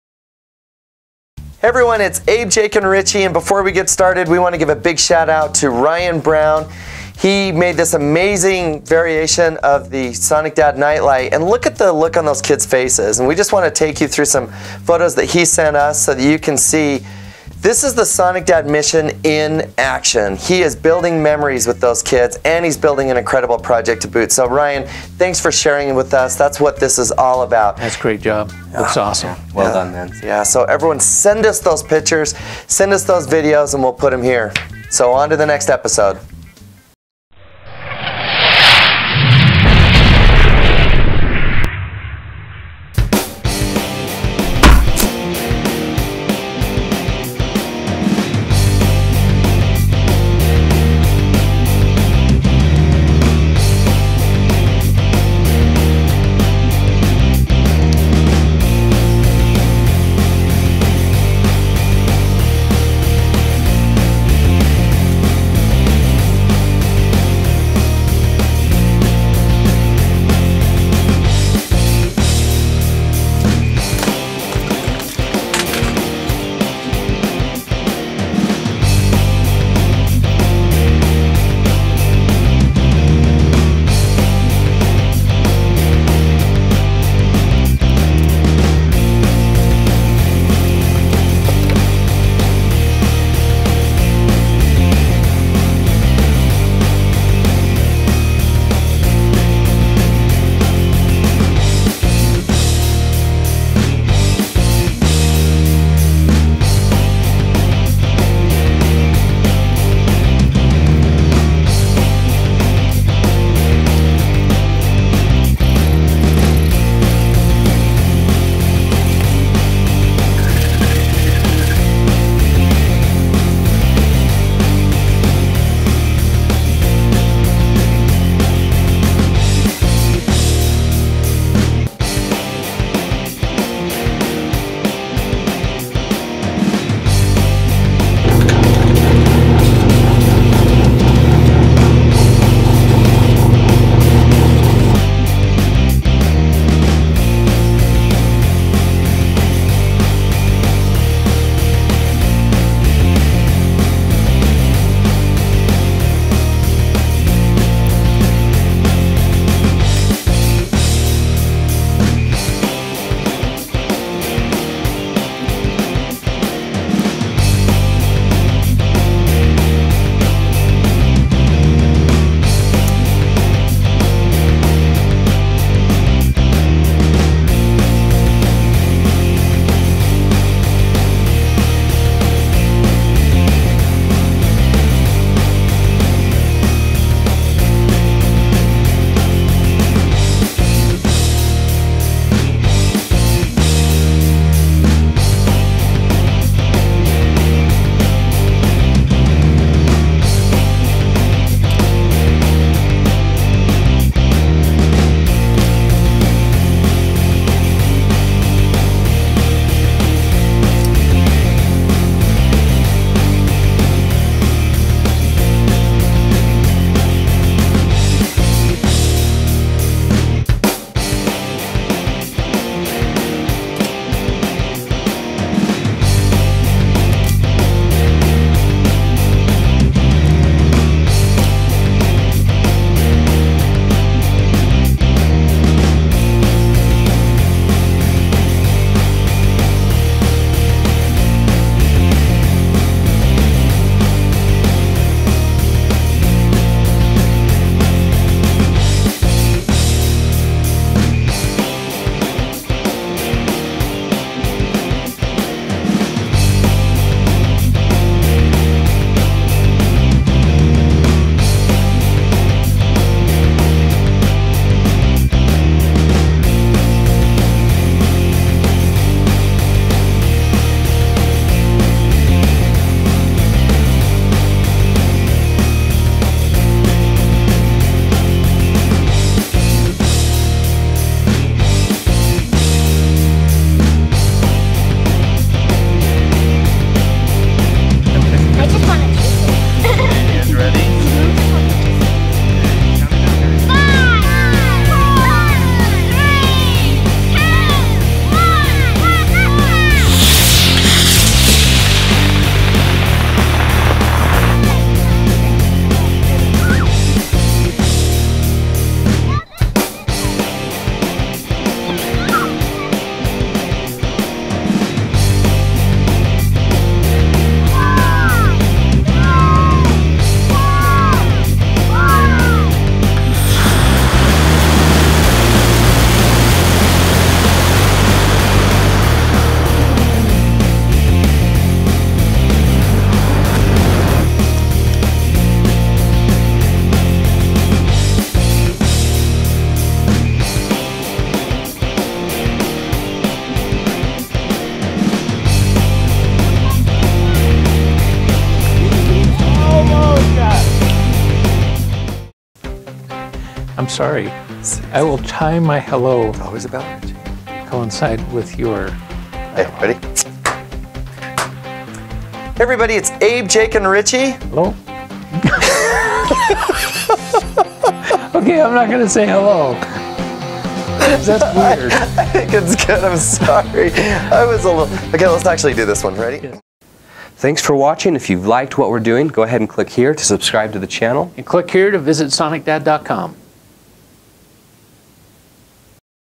The TKOR Randomizer Rocket: SonicDad Project #70
In this video we join forces with our good friend Grant Thompson, The King Of Random, to bring you an amazing flying model rocket. Made from less than $10 in materials, the TKOR Randomizer Rocket flies over 1,000 feet. This is a true design collaboration that has produced a flying model rocket unlike any other we've seen. We just know you're going to love it. Enjoy.